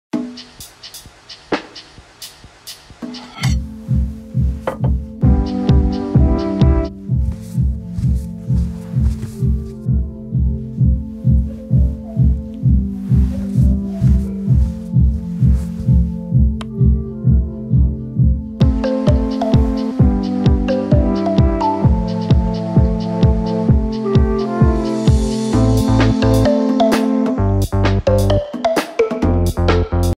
How to duplicate music from one room to another | IKEA Australia
Learn how to duplicate music from one room to another.